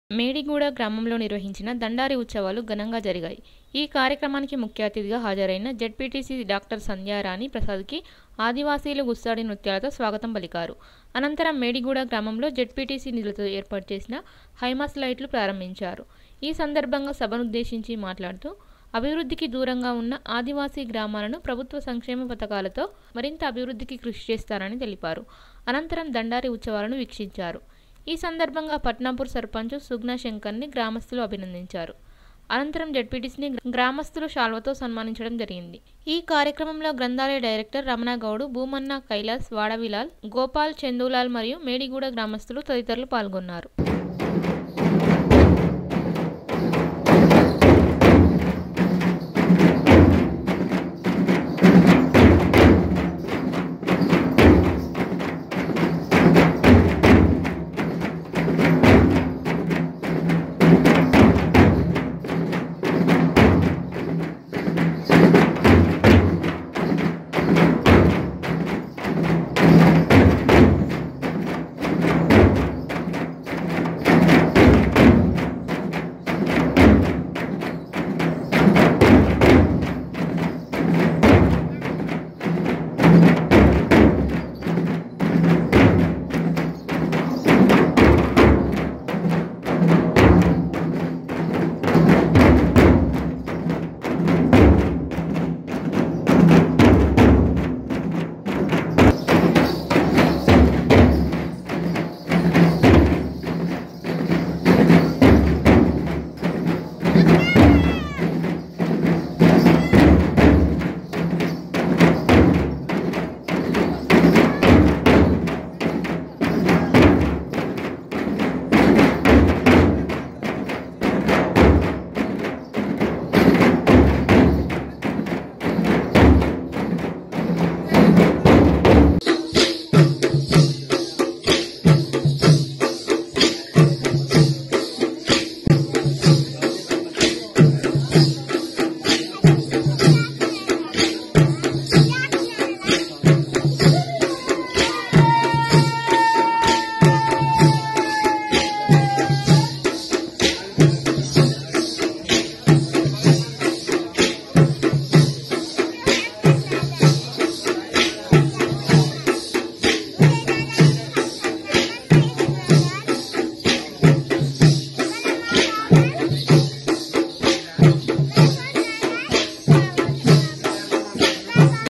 ఆదివాసి గ్రామాలకు అభివృద్ధికి సహకరిస్తా .....జెడ్పిటిసి డాక్టర్ సంధ్యారాణి ప్రసాద్ .
మేడిగూడ గ్రామంలో నిర్వహించిన దండారి ఉత్సవాలు ఘనంగా జరిగాయి ఇట్టి కార్యక్రమానికి ముఖ్య అతిథిగా హాజరైన జడ్పిటిసి గౌరవా డాక్టర్ సంధ్యారాణి ప్రసాద్ గారి కి ఆదివాసులు గుస్సాడీ నృత్యాలతో స్వాగతం పలికారు.అనంతరం మేడిగూడ గ్రామంలో జడ్పిటిసి నిధులతో ఏర్పాటు చేసిన హైమాస్ లైట్లు ప్రారంభించారు .ఈ సందర్భంగా సభను ఉద్దేశించి మాట్లాడుతూ అభివృద్ధికి దూరంగా ఉన్న ఆదివాసి గ్రామాలను ప్రభుత్వ సంక్షేమ పథకాలతో మరింత అభివృద్ధి కృషి చేస్తానని తెలిపారు అనంతరం దండారి ఉత్సవాలను వీక్షించారు ఈ సందర్భంగా పట్నాపూర్ సర్పంచ్ సుగుణ శంకర్ గారిని గ్రామస్తులు  అభినందించారు.అనంతరం జడ్పిటిసి గారిని గ్రామస్తులు శాలువతో సన్మానించడం జరిగింది. ఇట్టి కార్యక్రమంలో గ్రంధాలయ , డైరెక్టర్ రమణ గౌడ్ భూమన్న  కైలాస్ వాడవిలాల్ గోపాల్ చందులాల్ మరియు మేడీ గూడ గ్రామస్తులు తదితరులు పాల్గొన్నారు.